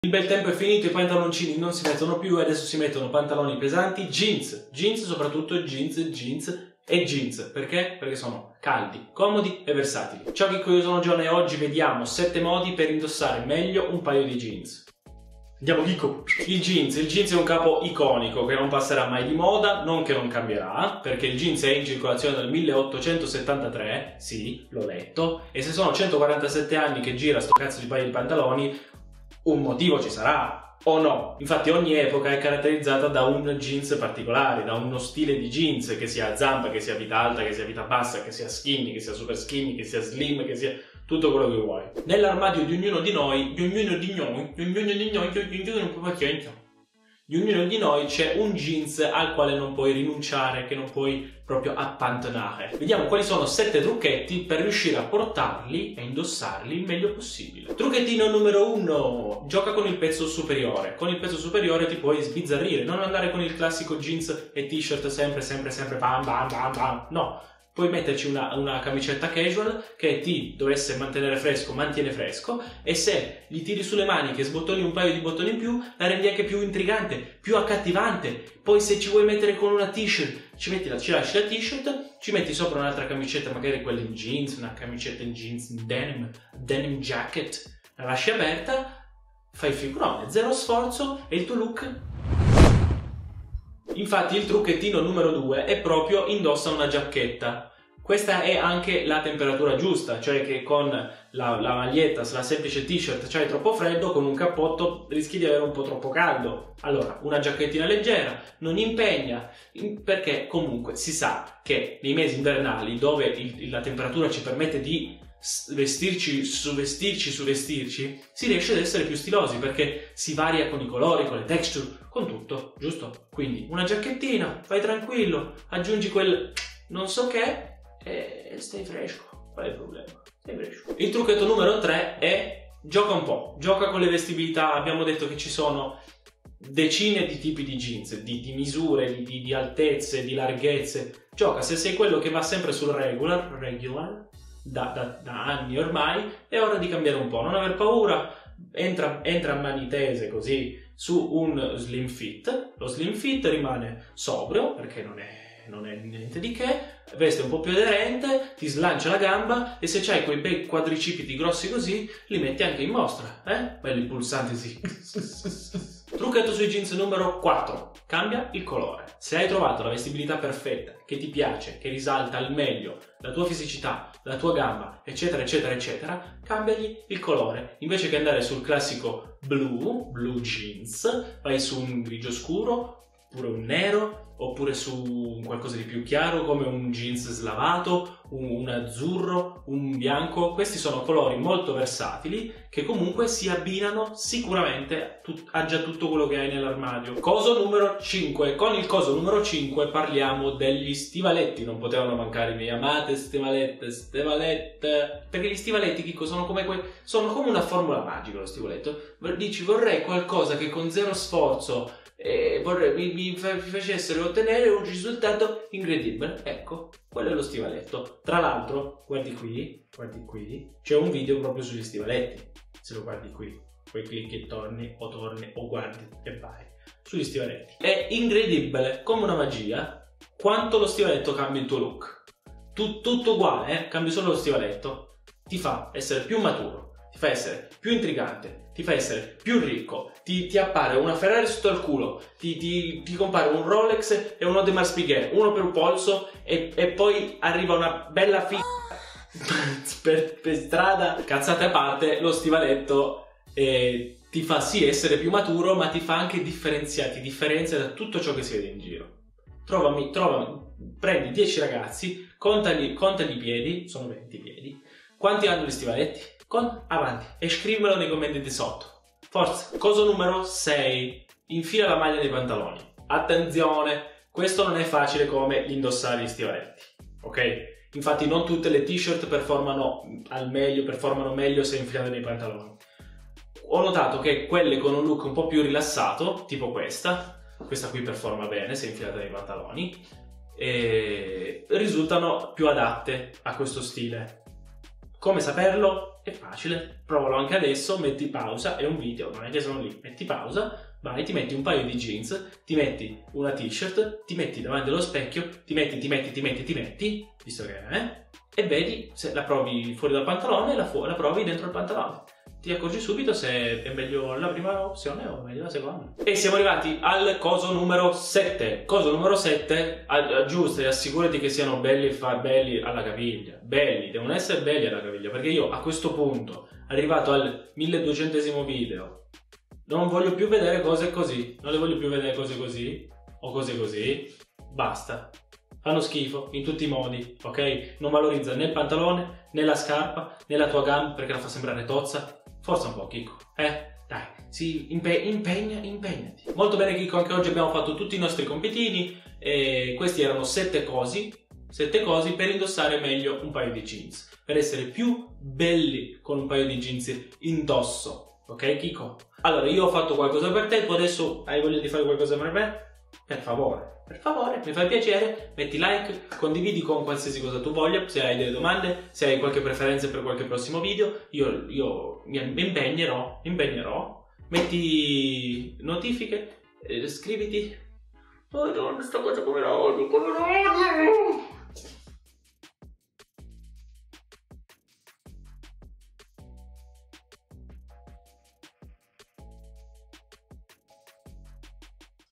JEANS OUTFIT : COME indossare i JEANS AL MEGLIO | Stile Uomo
Sicuramente hai almeno un paio di jeans nell'armadio, ma magari non stai sfruttando il potenziale di questo pantalone al massimo.
Vediamo quali sono i punti forti di questo storico pantalone, e come sfruttarli nel modo più stiloso possibile.